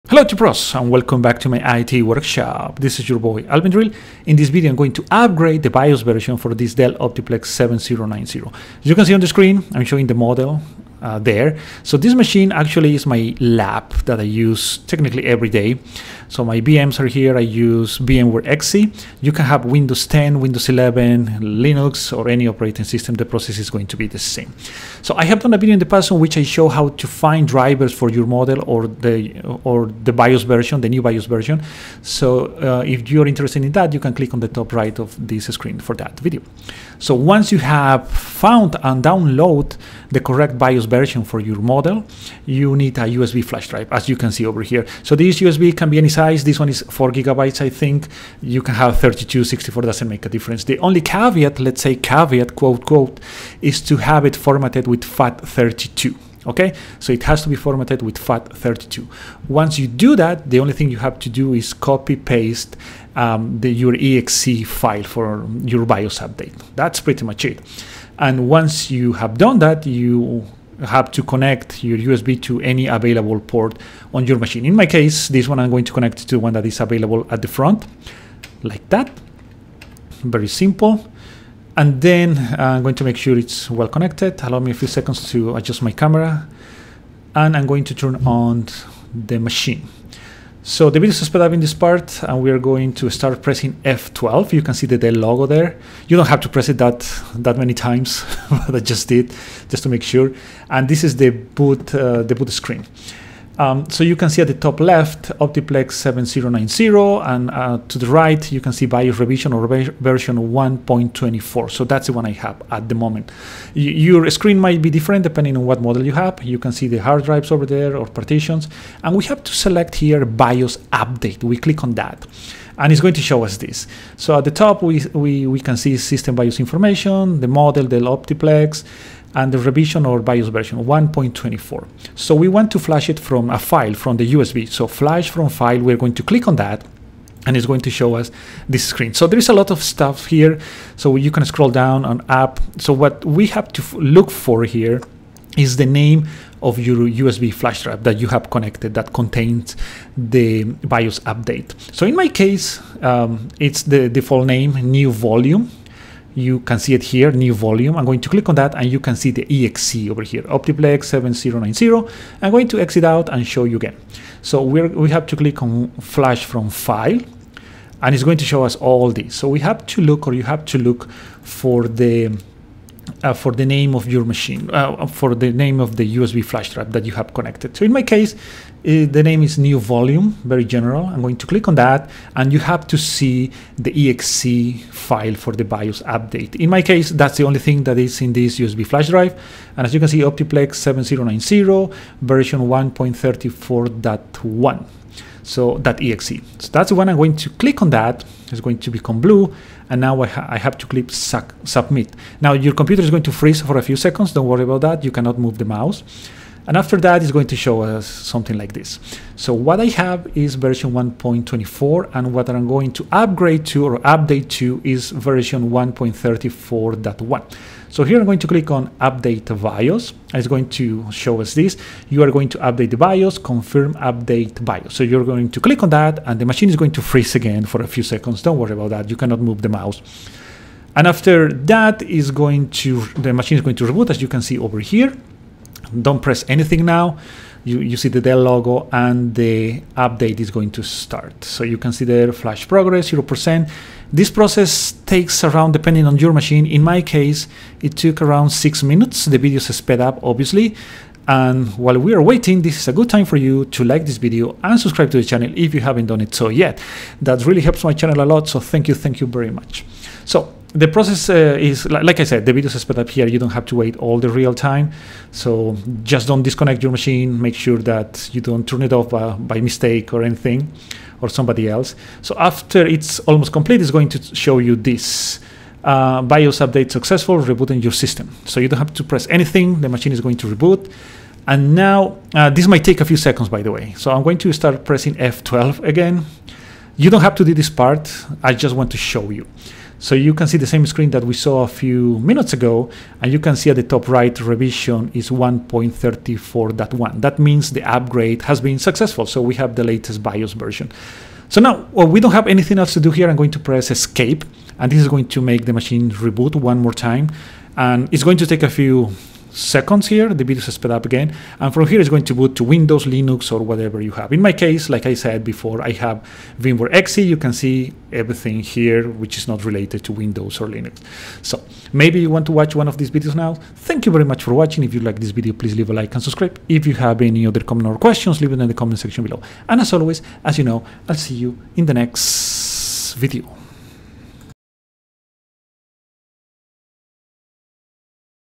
Dell Optiplex 7090 BIOS UPdate
Hello IT Pros, this is Alvendril! In this Video "Dell Optiplex 7090 BIOS UPdate".
 I show How to UPgrade the Bios version on a Dell Optiplex 7090. In order to do that the update file is needed + a USB Flash Drive formatted using FAT 32.  The process took around 6 minutes to complete. Hope it is useful!

-"Super Thanks" button below the tittle of every video

📌Have some time to become better? Here're some Books I like:
Rendezvous with Rama